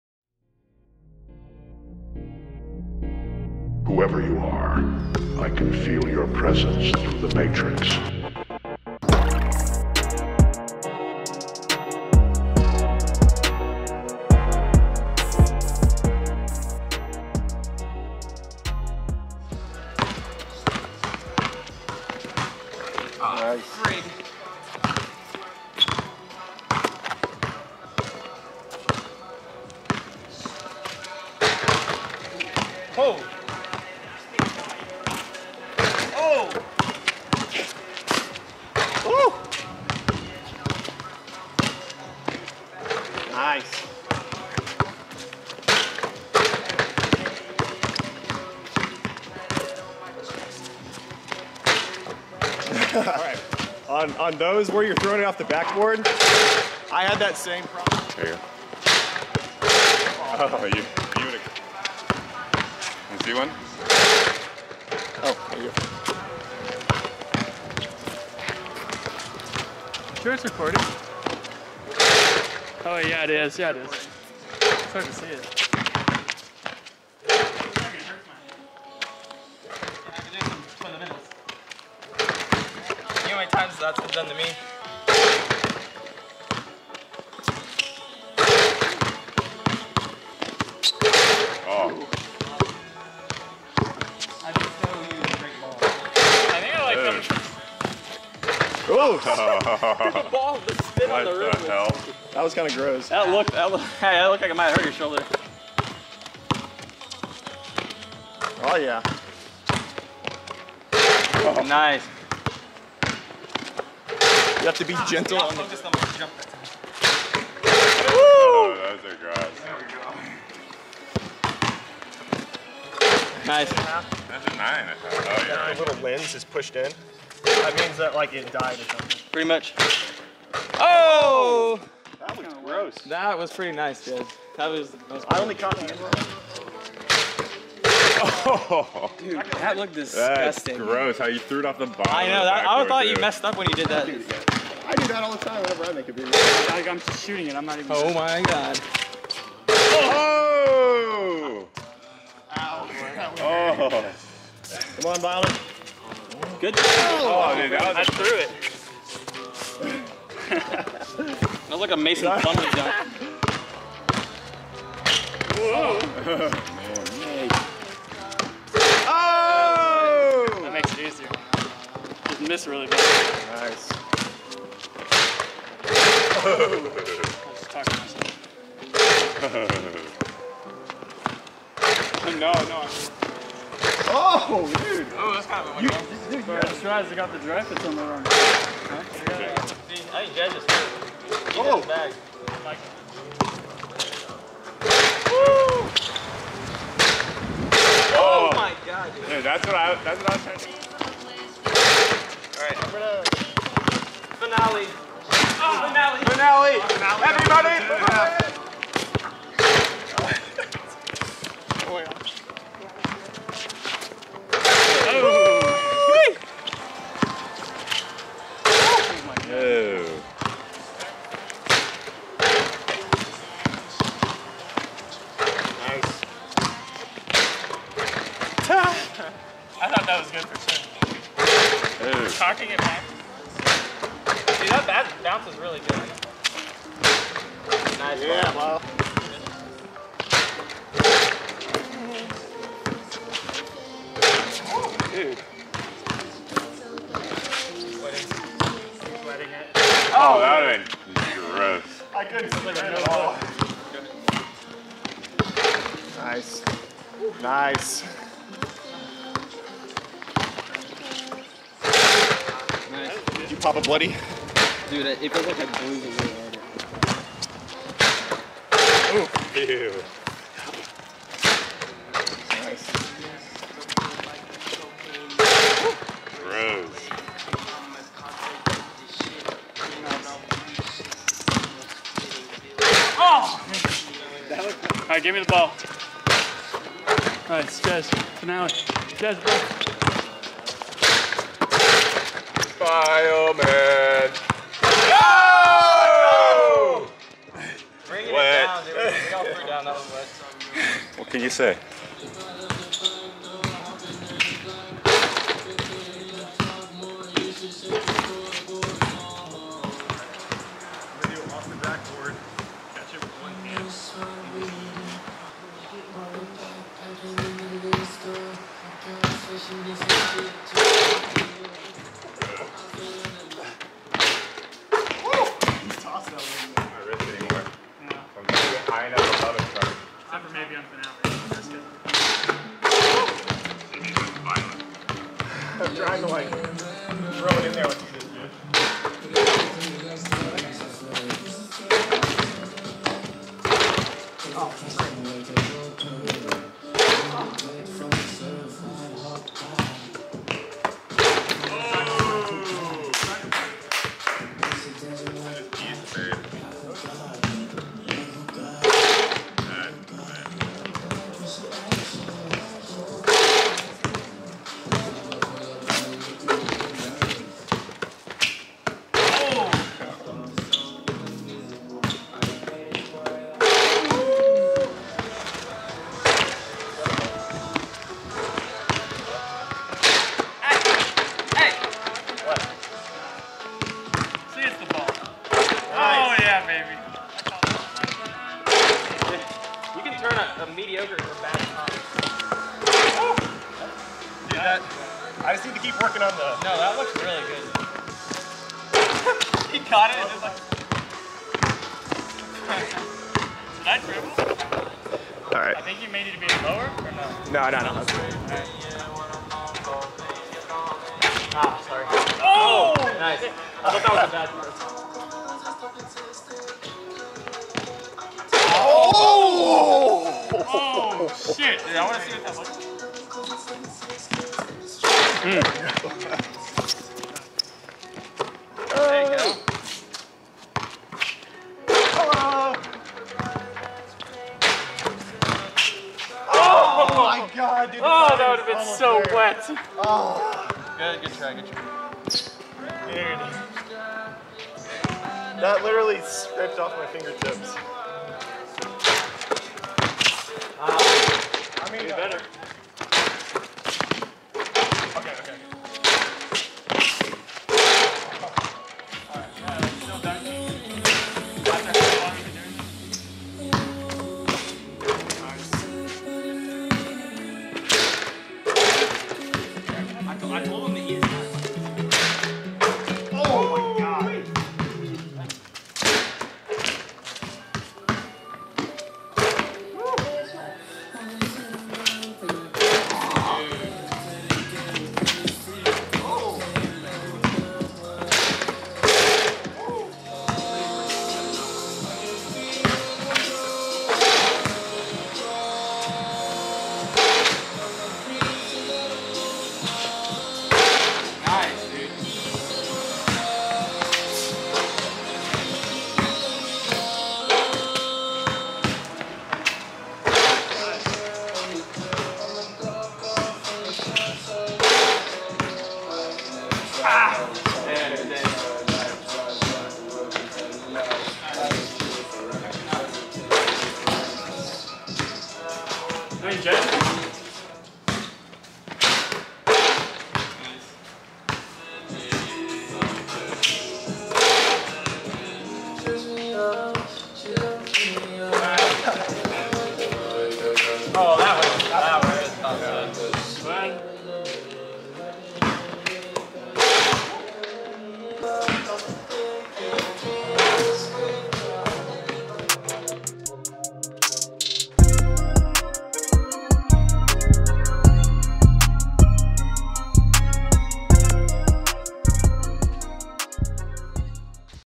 FULL DUNK SESSION #6┃LOW RIM DUNKS
A little low rim session with my friends.
Uploading 2-3 dunk sessions a week to track my progress, as well as lifting legs to increase my vert. Any questions or comments, leave them below and I'll get back to you. Appreciate it!